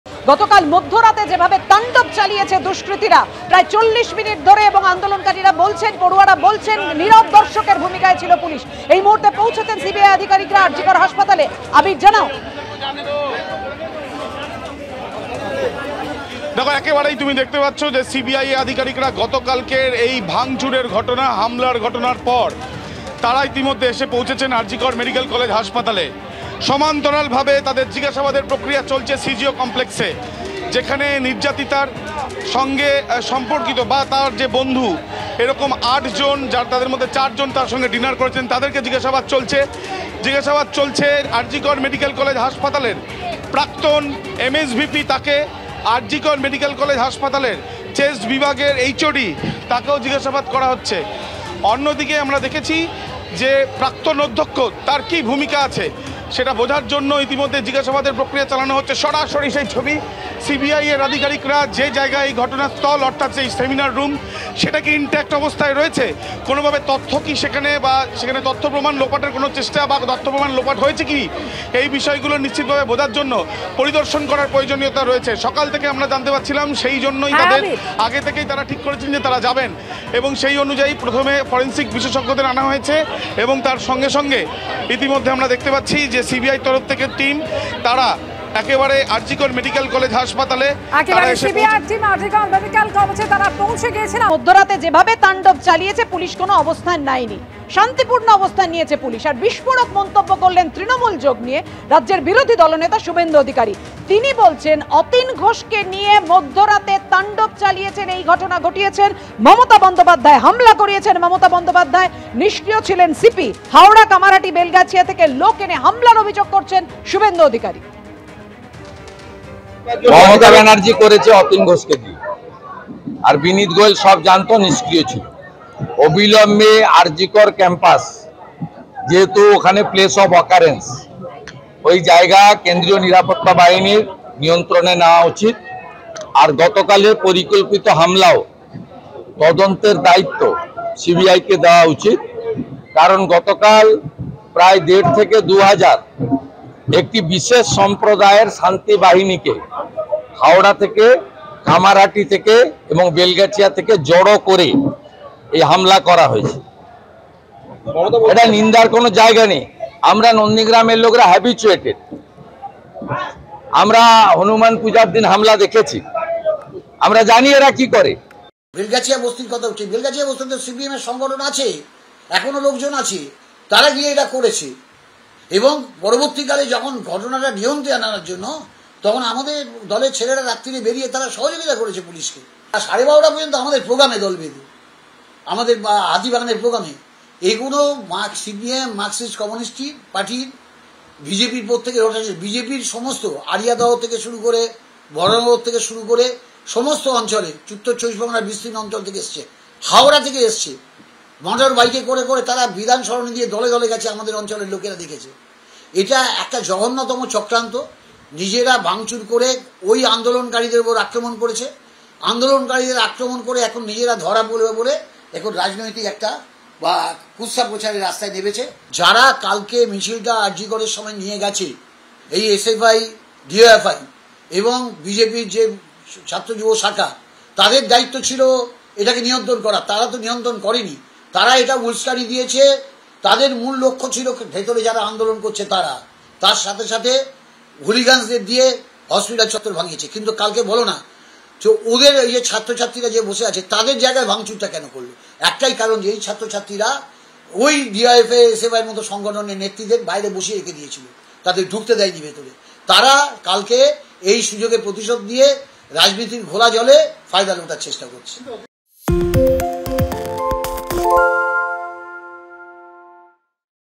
RG Kar Hospital: আরজি করে মধ্যরাতে তাণ্ডব, হাসপাতালে হাজির সিবিআই- এর দল | ABP Ananda Live
ABP Ananda Live: বুধবার মধ্যরাতে প্রায় ৪০ মিনিট ধরে তাণ্ডব চলেছে আরজি করে হাসপাতালে। নীরব দর্শকের ভূমিকায় ছিল পুলিশ। আজ সন্ধ্যায় আরজি কর হাসপাতালে যায় সিবিআই- এর তদন্তকারী দল। অন্যদিকে সিজিও কমপ্লেক্সেও মৃতা চিকিৎসকের সঙ্গে সম্পর্কিত বন্ধু মোট ৮ জনকে জিজ্ঞাসাবাদ করছে সিবিআই- এর তদন্তকারী আধিকারিকরা। এছাড়াও প্রাক্তন অধ্যক্ষের ভূমিকা বোঝার জন্য চলছে জিজ্ঞাসাবাদ। এর পাশাপাশি আরজি কর হাসপাতালে প্রাক্তন এমএসভিপি এবং চেস্ট বিভাগের প্রধানকেও জিজ্ঞাসাবাদ করা হচ্ছে। সেমিনার হল থেকে তথ্যপ্রমাণ লোপাট হয়েছে কিনা তা পরিদর্শনে এসেছে সিবিআই- এর দল। এসেছে ফরেন্সিক টিমও।